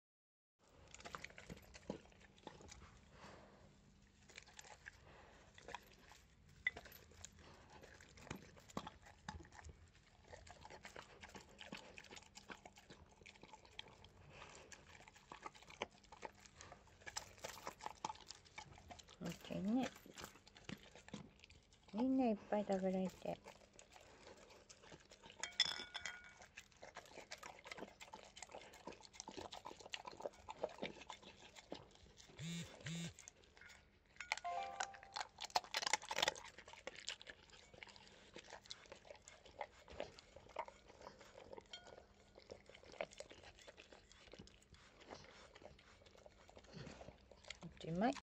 ラブラドール ロン おうち記念日のケーキを食す
わんこのパンとケーキ専門店 ベーカリーてぃす さんのシフォンケーキにトッピングしてみた。製作時間、約10分
完食時間、約40秒♪
クッキーも良い音させて、美味しいは、うれしいね。今日も完食ありがとう♪

▼犬ごはんblog  ロンさんの、健康管理手帳▼
ronnnookala.blog.jp